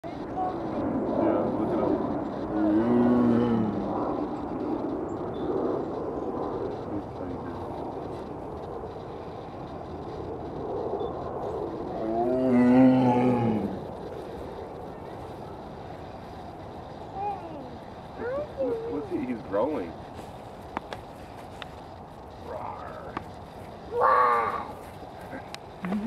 Tiger Roar
A sound I hear all the time while at the zoo, but have never caught on camera! :)

Minnesota Zoo